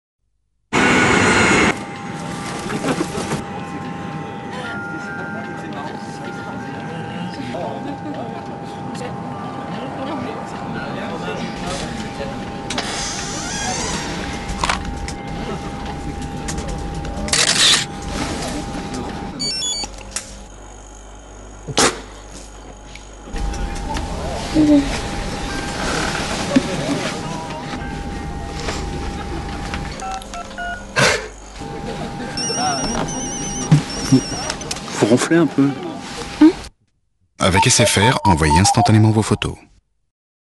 Говори лучше
Каждый знает, что в самолете нужно отключать сотовый. Кроме этого парня.